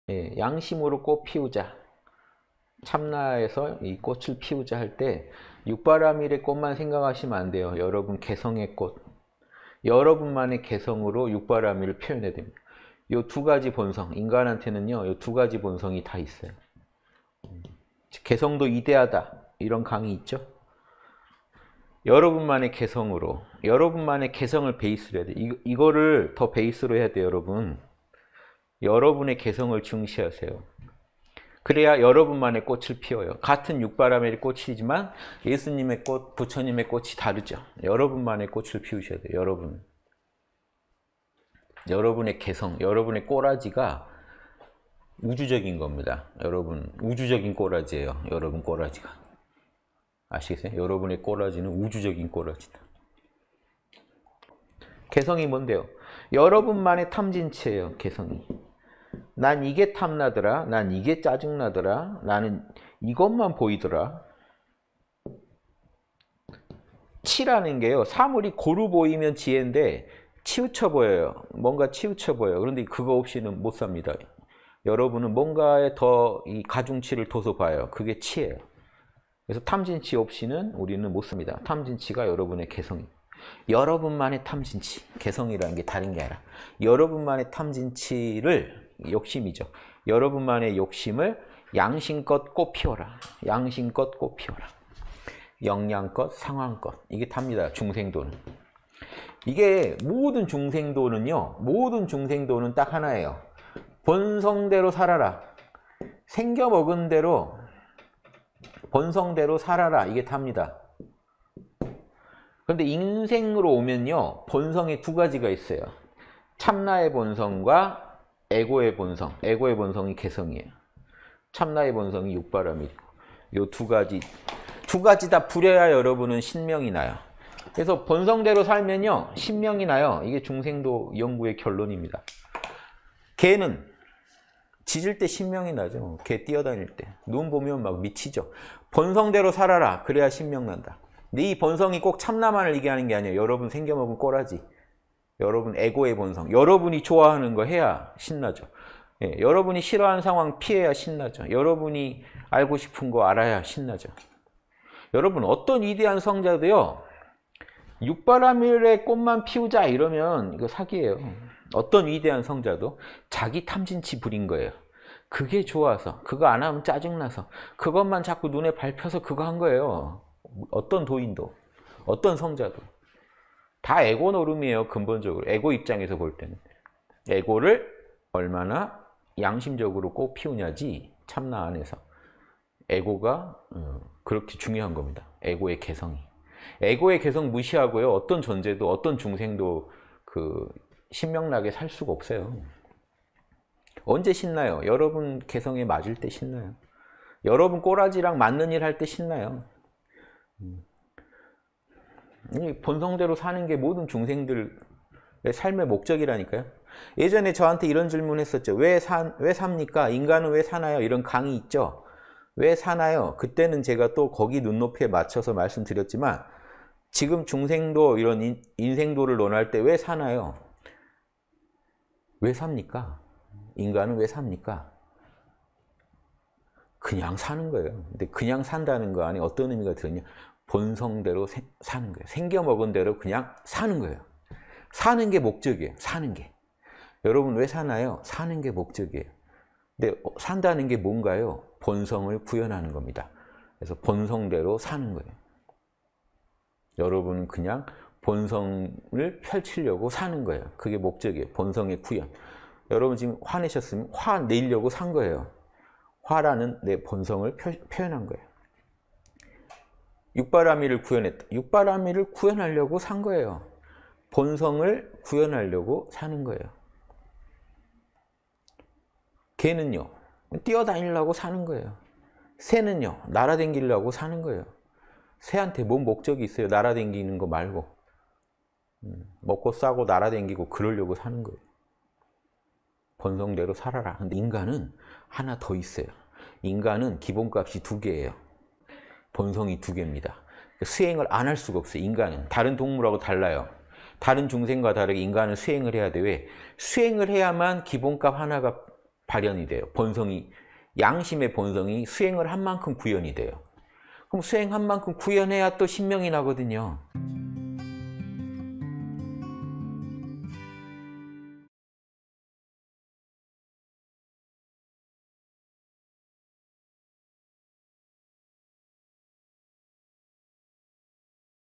[3분 인문학] 인간은 왜 사는가? "개성의 꽃을 피워라"
- (211018 양심톡톡) 인간은 왜 사는가? "개성의 꽃을 피워라"
양심으로 꽃피우자!
참나에서 이 꽃을 피우자 할 때,
6바라밀의 꽃만 생각하시면 안 돼요.
여러분 개성의 꽃,
여러분만의 개성으로 6바라밀을 표현해야 됩니다.
이 두 가지 본성, 인간한테는요, 이 두 가지 본성이 다 있어요.

'개성도 이데아다!' 이런 강의 있죠?
여러분만의 개성으로, 여러분만의 개성을 베이스로 해야 돼요.
이것을 더 베이스로 해야 돼요, 여러분.
여러분의 개성을 중시하세요.
그래야 여러분만의 꽃을 피워요.
같은 6바라밀이 꽃이지만 예수님의 꽃, 부처님의 꽃이 다르죠.

여러분만의 꽃을 피우셔야 돼요, 여러분은.
여러분의 개성, 여러분의 꼬라지가 우주적인 겁니다.
여러분 우주적인 꼬라지예요, 여러분 꼬라지가. 아시겠어요?
여러분의 꼬라지는 우주적인 꼬라지다.
개성이 뭔데요? 여러분만의 탐진치예요, 개성이.

"난 이게 탐나더라." "난 이게 짜증 나더라."
"나는 이것만 보이더라."
'치'라는 게요, 사물이 고루 보이면 지혜인데,
치우쳐 보여요. 뭔가 치우쳐 보여요.
그런데 그거 없이는 못 삽니다.
여러분은 뭔가에 더 가중치를 둬서 봐요.
그게 '치'예요. 그래서 탐진치 없이는 우리는 못 삽니다.

탐진치가 여러분의 개성, 여러분만의 탐진치예요.
개성이라는 게, 다른 게 아니라,
여러분만의 탐진치를, 욕심이죠.
"여러분만의 욕심을 양심껏 꽃피워라! 양심껏 꽃피워라!
역량껏 상황껏!" 이게 다입니다, 중생도는.
이게 모든 중생도는요, 모든 중생도는 딱 하나예요.
"본성대로 살아라!" "생겨먹은 대로 본성대로 살아라!"
이게 다입니다.

그런데 인생으로 오면요,
본성이 두 가지가 있어요, 참나의 본성과, 에고의 본성.
에고의 본성이 개성이에요. 참나의 본성이 6바라밀.
두 가지 다 부려야 여러분은 신명이 나요.
그래서 본성대로 살면요, 신명이 나요.
이게 중생도 연구의 결론입니다.
개는 짖을 때 신명이 나죠.
개 뛰어다닐 때 눈 보면 막 미치죠.
본성대로 살아라, 그래야 신명 난다.

네 본성이 꼭 참나만을 이야기하는 게 아니에요.
여러분 생겨먹은 꼬라지,
여러분 에고의 본성, 여러분이 좋아하는 거 해야 신나죠.
여러분이 싫어하는 상황을 피해야 신나죠.
여러분이 알고 싶은 거 알아야 신나죠.
여러분 어떤 위대한 성자도요,
"6바라밀의 꽃만 피우자!" 이러면 사기예요.

어떤 위대한 성자도 자기 탐진치 부린 거예요.
그게 좋아서, 그거 안 하면 짜증 나서,
그것만 자꾸 눈에 밟혀서, 그거 한 거예요.
어떤 도인도, 어떤 성자도, 다 에고 놀음이에요,
근본적으로, 에고 입장에서 볼 때는.
에고를 얼마나 양심적으로 꽃피우냐지, 참나 안에서,
에고가 그렇게 중요한 겁니다, 에고의 개성이.

에고의 개성 무시하고요,
어떤 존재도, 어떤 중생도, 신명 나게 살 수가 없어요.
언제 신나요? 여러분 개성에 맞을 때 신나요.
여러분, 꼬라지랑 맞는 일할 때 신나요.
본성대로 사는 게, 모든 중생들 삶의 목적이라니까요?
예전에 저한테 이런 질문 했었죠?

"왜 삽니까?" "인간은 왜 사나요?"
이런 강의 있죠? "왜 사나요?",
그때는 제가 또 거기 눈높이에 맞춰서 말씀드렸지만,
지금 중생도 이런 인생도를 논할 때, "왜 사나요?"
왜 삽니까? 인간은 왜 삽니까?
그냥 사는 거예요.
그런데 그냥 산다는 거 안에 어떤 의미가 들어있느냐?

본성대로 사는 거예요.
생겨먹은 대로 그냥 사는 거예요.
사는 게 목적이에요, 사는 게.
여러분 왜 사나요? 사는 게 목적이에요.
산다는 게 뭔가요? 본성을 구현하는 겁니다.
그래서 본성대로 사는 거예요.
여러분 그냥 본성을 펼치려고 사는 거예요.
그게 목적이에요, 본성의 구현.

여러분 지금 화내셨으니, 화내려고 산 거예요.
화라는 내 본성을 표현한 거예요.
6바라밀을 구현했다. 6바라밀을 구현하려고 사는 거예요.
본성을 구현하려고 사는 거예요.
개는요? 뛰어다니려고 사는 거예요.
새는요? 날아다니려고 사는 거예요.

새한테 무슨 목적이 있어요? 날아다니는 것 말고.
먹고, 싸고, 날아다니고, 그러려고 사는 거예요.
"본성대로 살아라!"
인간은 하나 더 있어요.
인간은 기본값이 두 개예요. 본성이 두 개입니다.
수행을 안 할 수가 없어요, 인간은.
다른 동물하고 달라요.

다른 중생과 다르게 인간은 수행을 해야 돼요.
왜? 수행을 해야만
기본값 하나가 발현이 돼요, 본성이.
양심의 본성이 수행한 만큼 구현이 돼요.
그럼 수행한 만큼 구현해야 또 신명이 나거든요.